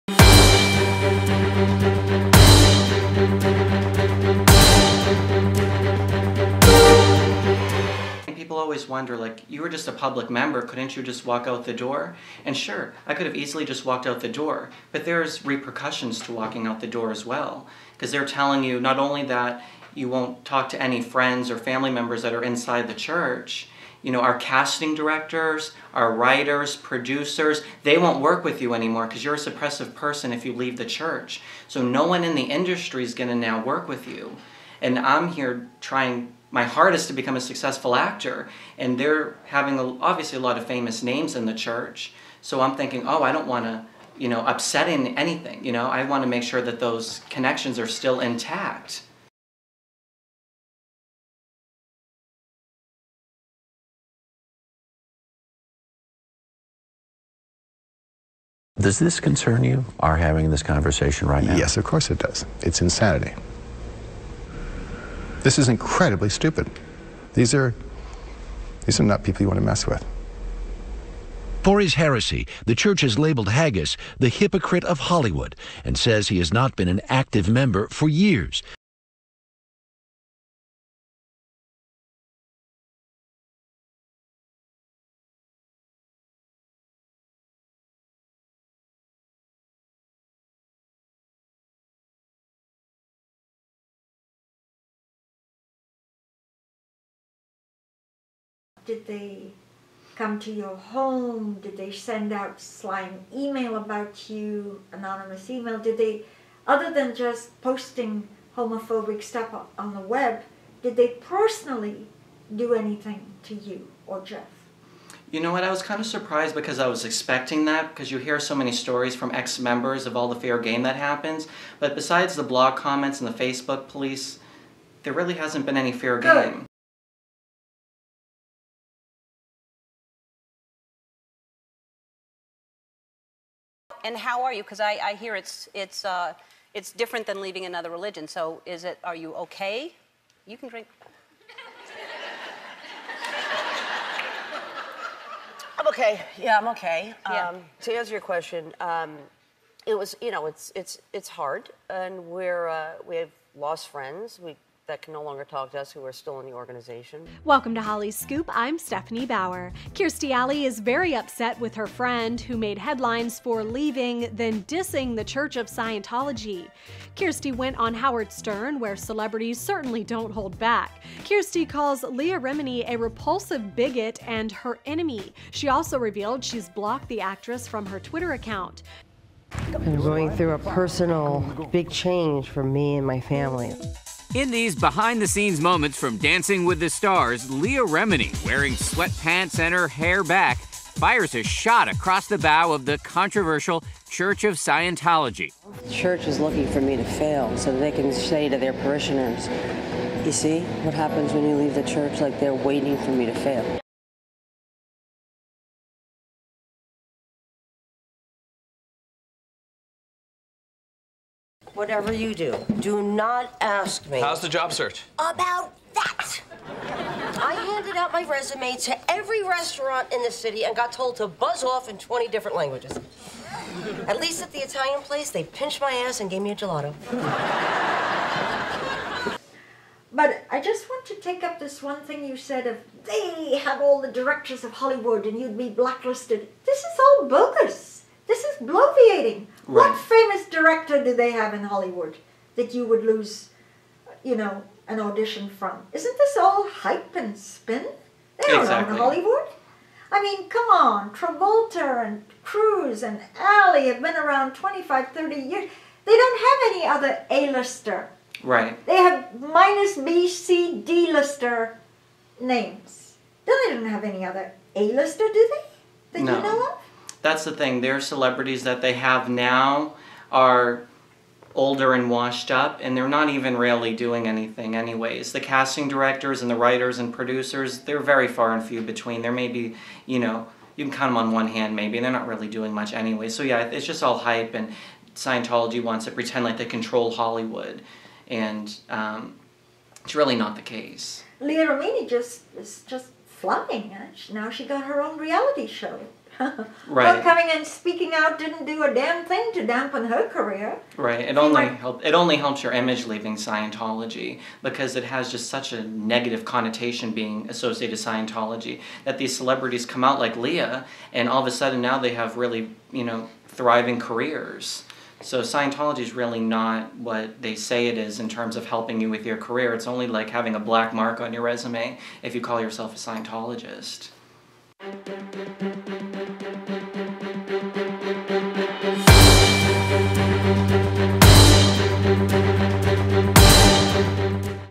Scientology Inc Celebrities ~~ The Spin, the Hype and Schmollywood Scientologists
Scientology Inc makes a repeating accusation of whistle blowers ~~ they are "self-obsessed" or "self-absorbed"
No one is more self-obsessed as an entity that the "church" of Scientology. Obsessed with "good public relations" it covers up child molestations, assault and battery, and other felonies from Law enforcement as a matter of routine.
Luring in celebrities is part of their strategy to have the status symbol of CELEBS.
See how the "church" responded to Paul Haggis and Leah Remini's departures.